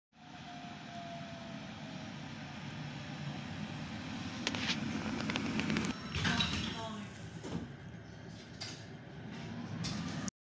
2. května 2020
Večerní spoj linky 26 na Želivského přijíždí s jedním vozem , protože bylo půl 12
T3 RP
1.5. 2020